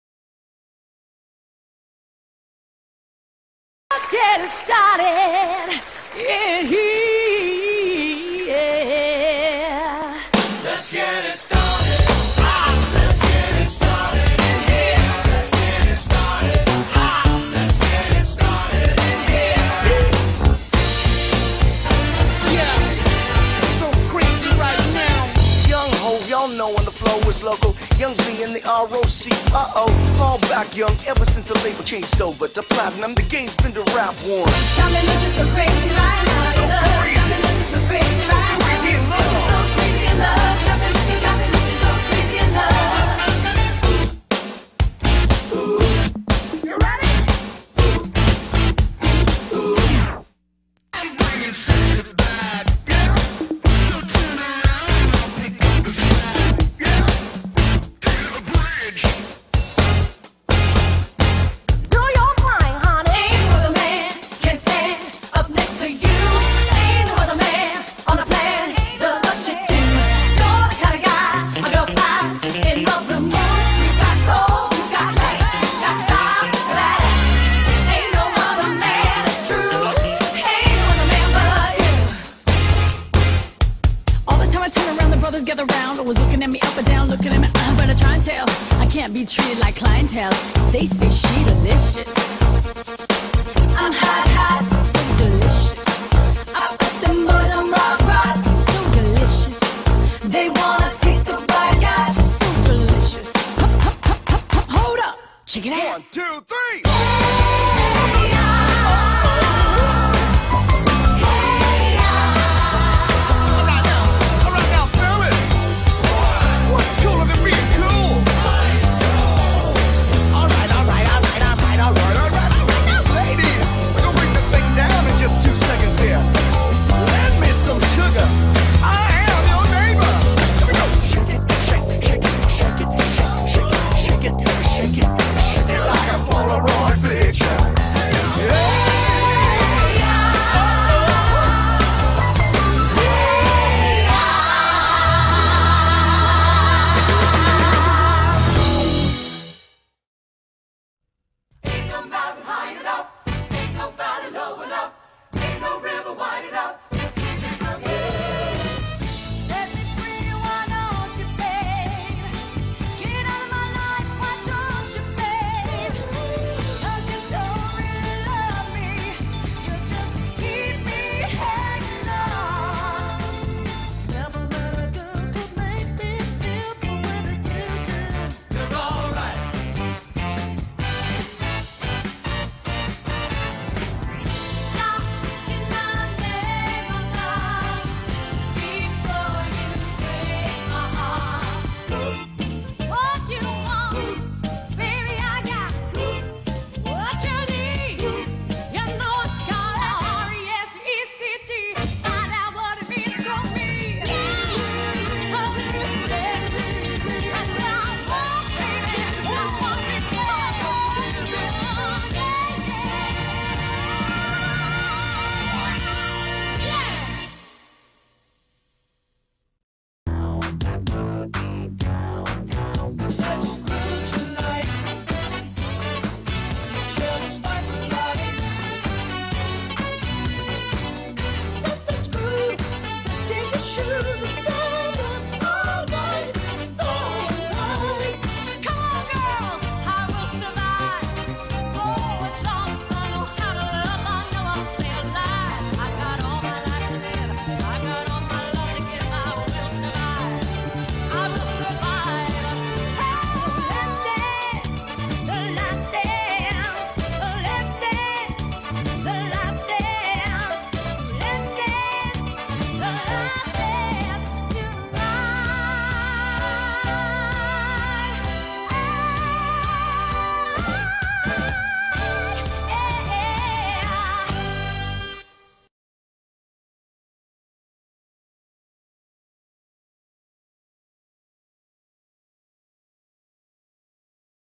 Z Cover Band Contemporary Motown R&B Disco Band
One of the best dance party variety bands for sure! They play all styles of music and every member sings.  Ask about Skip's band.